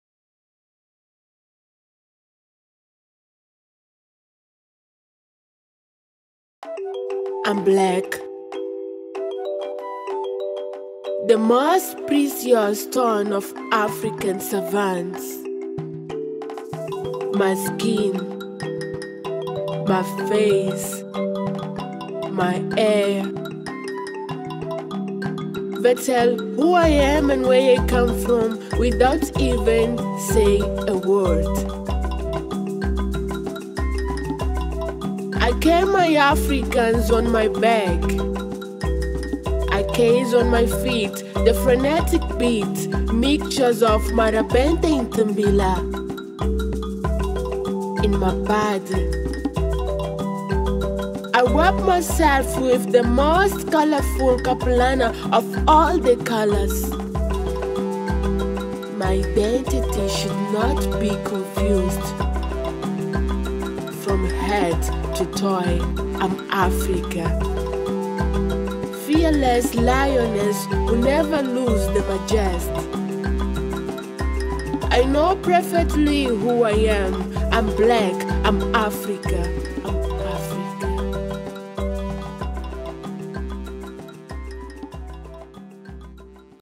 DigiPoems: African Identity
Mozambican poet Lorna Zita teamed up with Zimbabwean digital artist Tanya Prisca Njerere to create an amazing expression of art where poetry meets animation in the project DigiPoems. Enjoy this piece titled African Identity.

Creative Director & Lead Animator: Eugene Mapondera
Illustrator: Kimberly Khuleya
Animator: Tanyaradzwa Njerere
Art Director: Colin Meda
Sound Engineer & Music Producer: Innocent Masamba & Tatenda Jena
Project Managers: Keith Kuhudzai & Tanatsa Chawanda